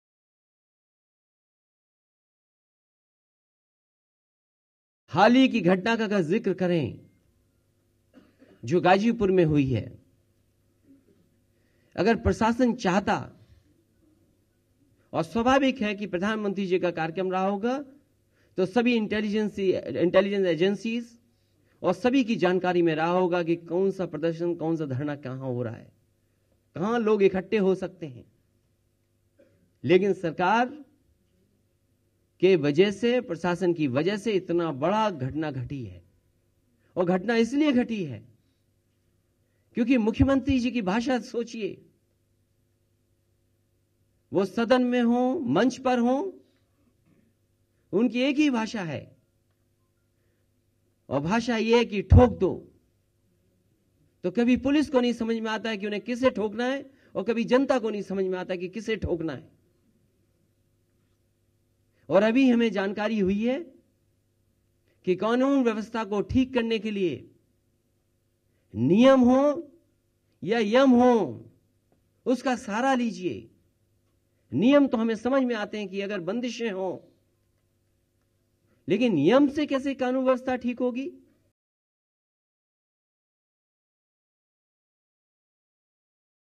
Ghazipur violence: CM Yogi’s speeches ignite such incidents, says Akhilesh Yadav
Lucknow (Uttar Pradesh), Dec 30 (ANI): Former Uttar Pradesh chief minister Akhilesh Yadav blamed Chief Minister Yogi Adityanath’s speech for igniting incidents like Ghazipur stone pelting, where a police constable was killed.